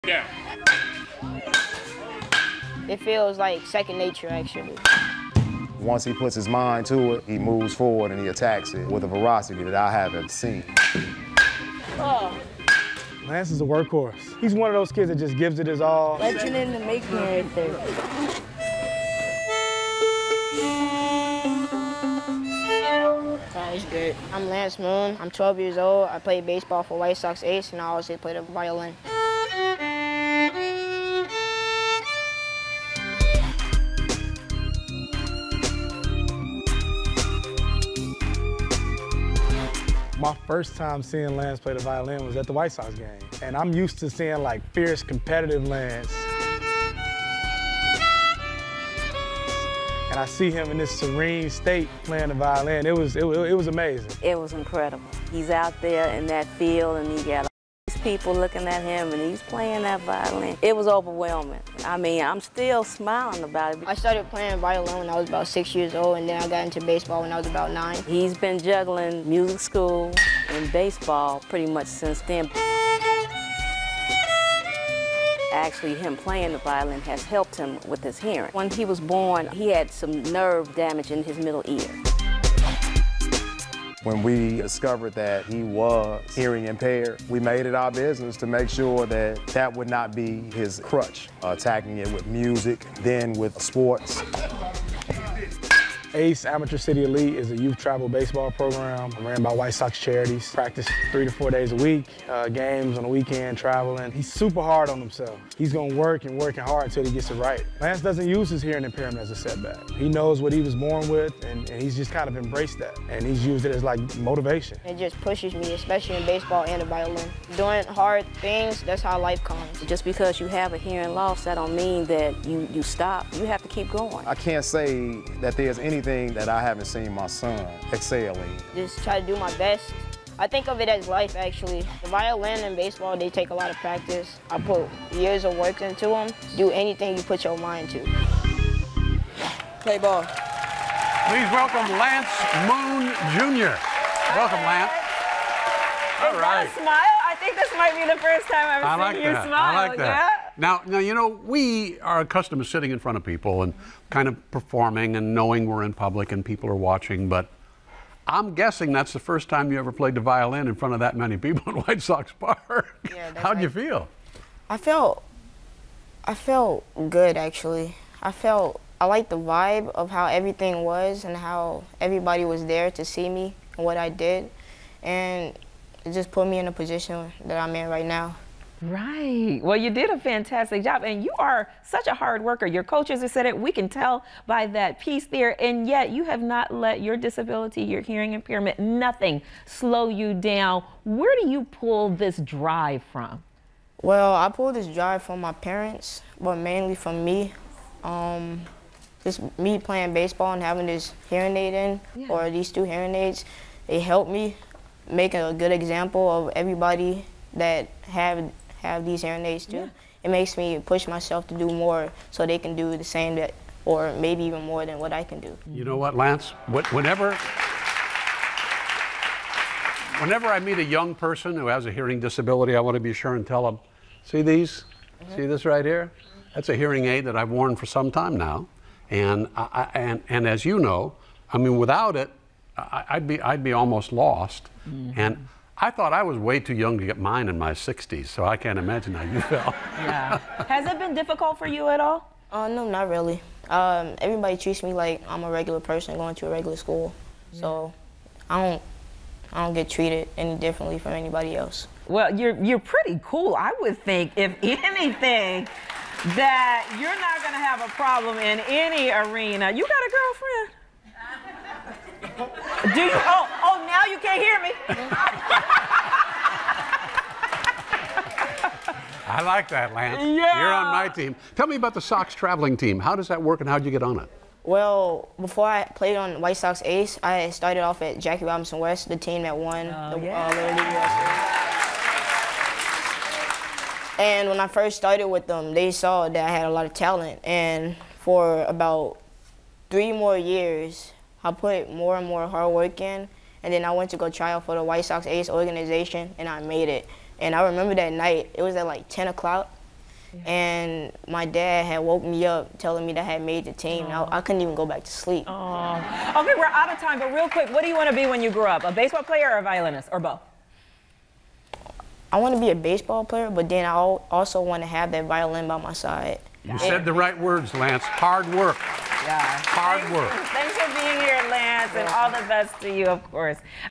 Amazing Kids: Lance Moon plays violin
In this week’s Amazing Kids, talented baseball and violin player, 12-year-old Lance Moon, shows us that even with a disability he will not be slowed down.